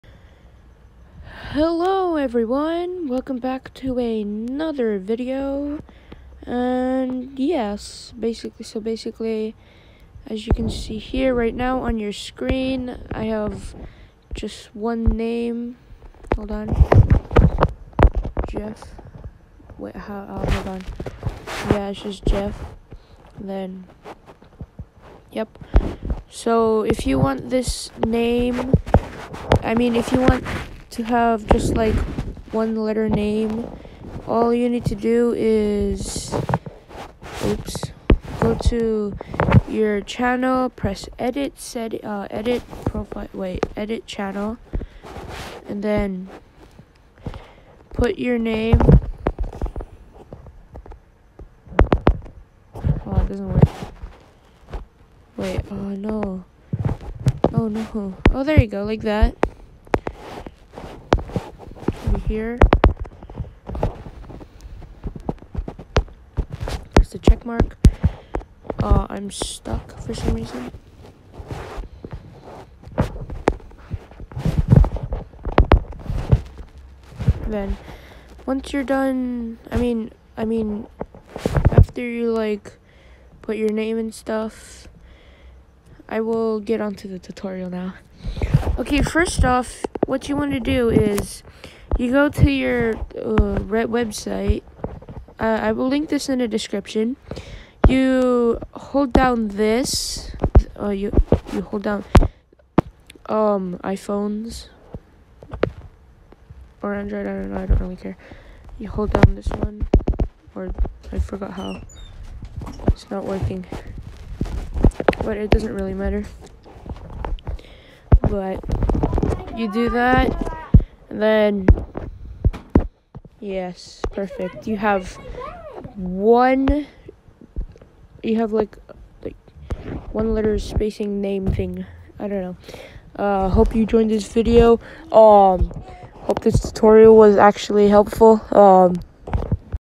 One word name tutorial! (Actual tutorial in desc)
So basically you go to your channel, press “Edit Channel” then you erase your name and put whatever name you want! After that you have a one word name!